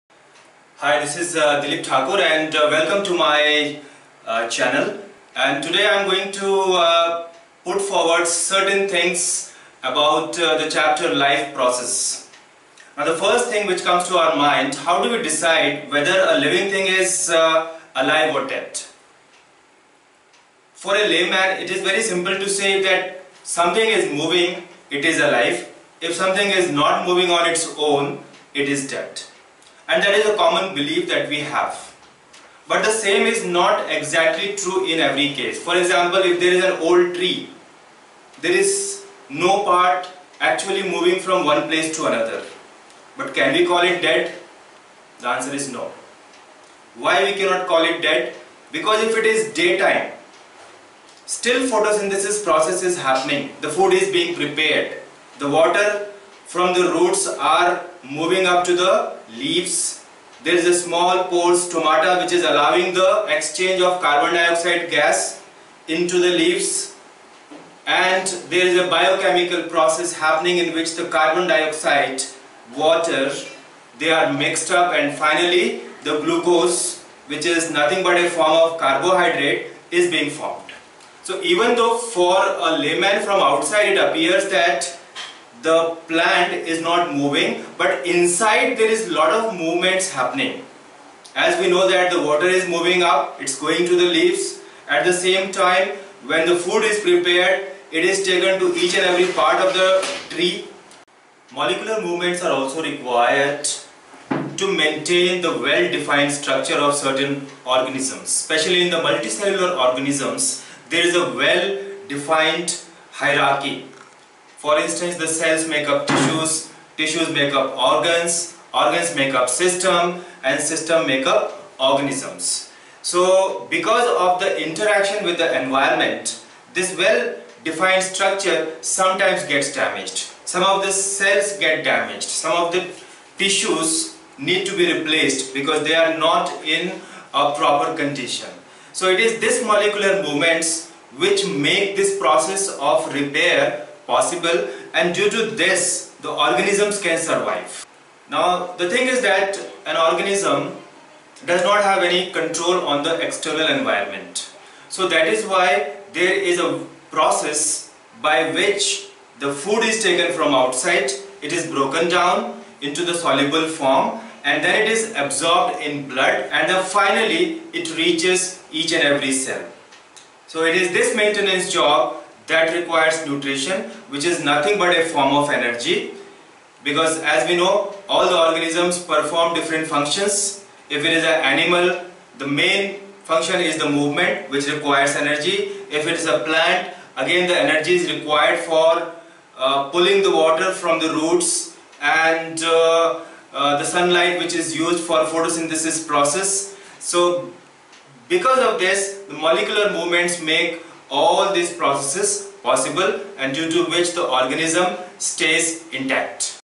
Life Process (Chapter 6 CBSE Tenth 10th Science)
This is the biology chapter of biology which deals with different life processes. First of all it deals with how to find out whether an organism is living or dead or alive. For this molecular movement is required. Even for the maintenance, molecular movement is needed. In plants which don't move from one place to another there is molecular movement in the form of water and food.
For upkeep and growth, food is taken from outside and is subjected to oxidation and reduction for breaking them down into simpler food for absorption in blood.
Multi cellular organisms have well-defined structure and the nutrition is important to maintain them well.